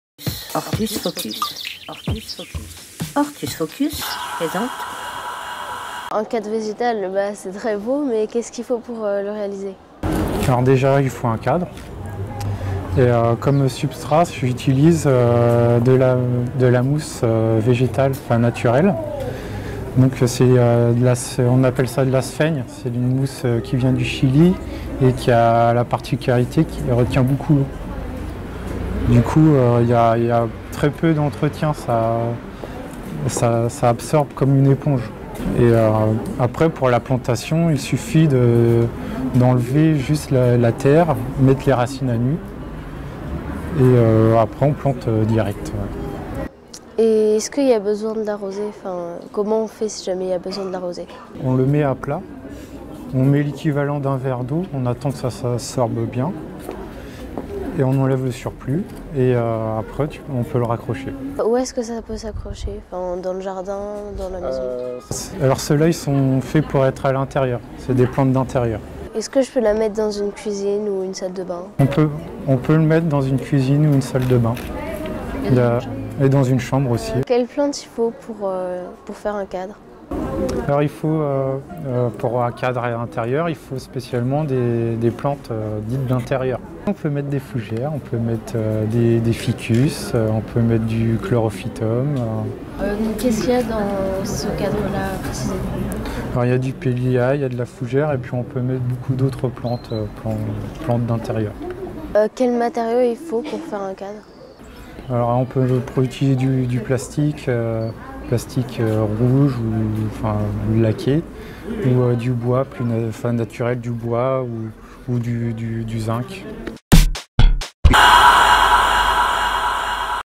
Créations végétales : les cadres végétaux
Le cadre végétal questionné par Marie-Nour et commenté par Thibaud Simolak. Retrouvez l'article sur le magazine Hortus Focus :